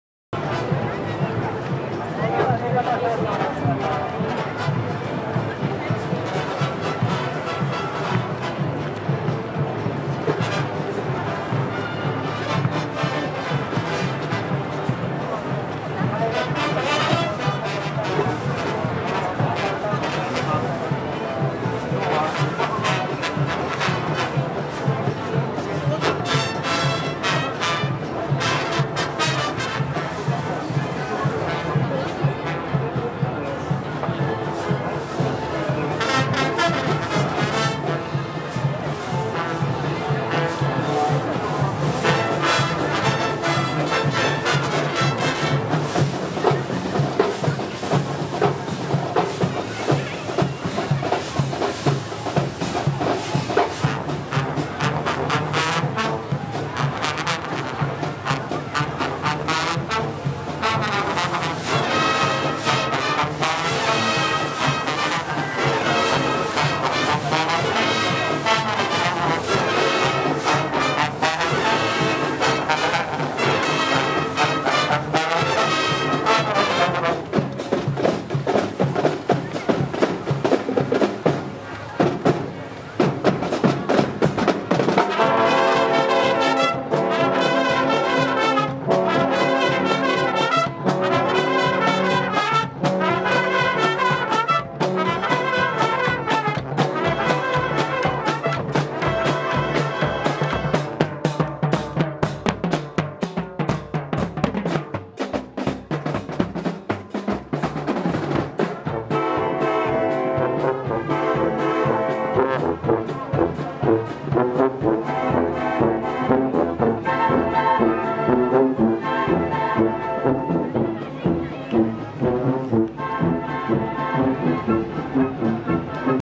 横浜創英中学・高等学校マーチングバンド・[SUPER 06]・2018ザよこはまパレード
2018年5月3日（木・祝）　66th ザ　よこはまパレード
横浜開港記念みなと祭　国際仮装行列